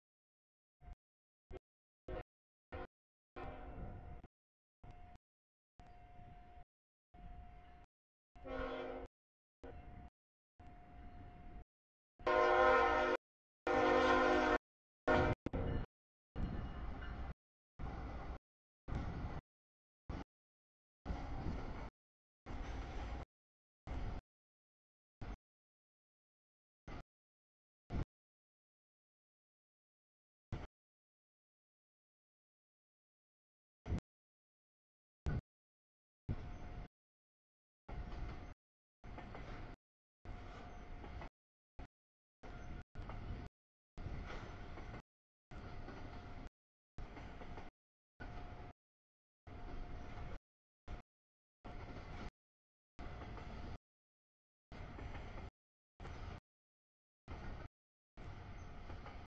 3- 12-2019 07:48 nb stack CSX 3276, 389 Q157
00:22 nb stack CSX Q167
01:05 nb stack trailers CSX Q009
01:24 nb stack CSX Q159
03:09 sb tanks CSX 789 K678
06:29 sb nysw cut, mix CSX Q409
06:41 nb mix, green trash CSX Q438
06:55 nb stack trailers CSX Q001
07:16 nb nysw cut, auto racks, epic trash CSX 3005 DPU UP 6188 Q410
07:33 nb stack trailers CSX 3202 BNSF 7052 Q003
07:48 nb stack CSX 3276, 389 Q157
08:30 nb stack trailers CSX 3182, 922 Q007
09:16 sb stack trailers CSX 708, 5276 Q010
09:35 sb stack CSX 3439 DPU 3204 Q160
11:21 nb mic CSX 881, CN 5456, CSX 5291, 5122, 712, 508, 235 Q190
13:13 nb mix CSX 4708, 5281 Q434
13:33 nb auto rack CSX 4582, 3344 Q253
15:00 sb mix CSX 5217, 3320 Q439
15:25 sb tanks BNSF 6580, 8217 K160
15:51 nb auto rack NS 9457 CSX 8037 Q261
16:06 nb empty's UP'S 8603, 5867 K683
16:56 sb auto rack CSX 5377 Q262
18:18 sb stack CSX 956 DPU 3442 Q004
19:17 sb tanks BNSF 5021 BNSF 9109 CSX 59 K138